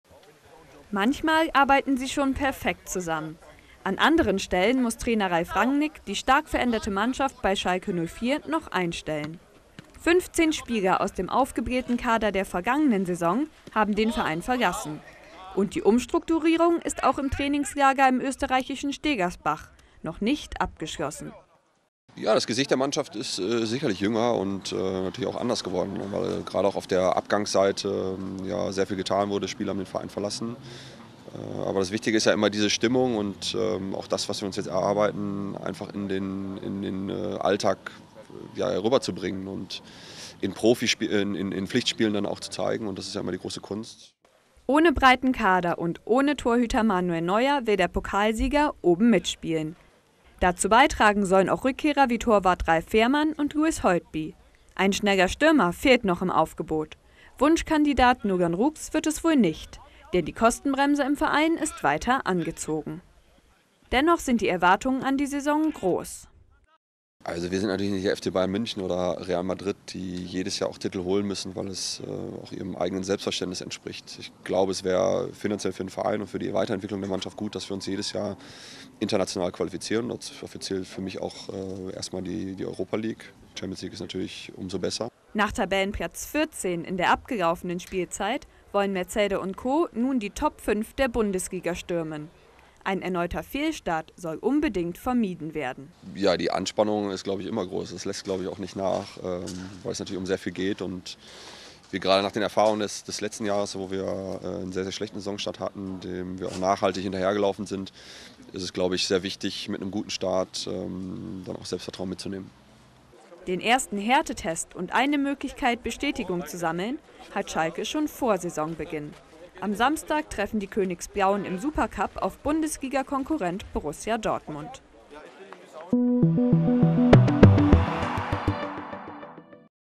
Schalke setzt sich unter Druck
Der Pokalsieger will nach der durchwachsenen vergangenen Saison in der neuen Spielzeit wieder angreifen. Nach weitreichenden Veränderungen im Kader muss Trainer Ralf Rangnick die Mannschaft von Schalke 04 aber erst einmal neu einstellen.